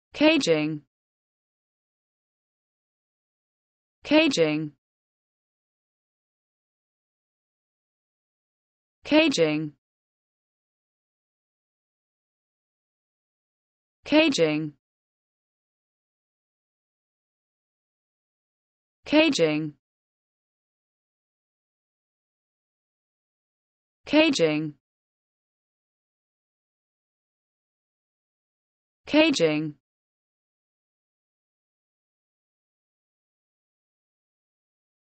the correct pronunciation of caging in English.
This video shows you how to pronunciation caging in English.